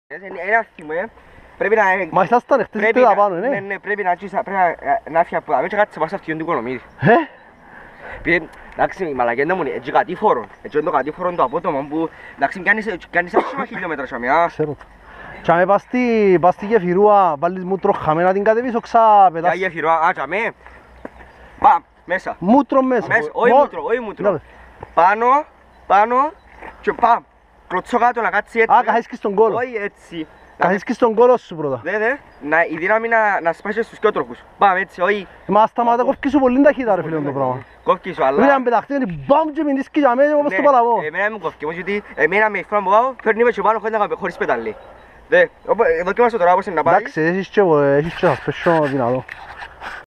Talking after a ride MPAM
Funny talk after a Stavrovouni ride.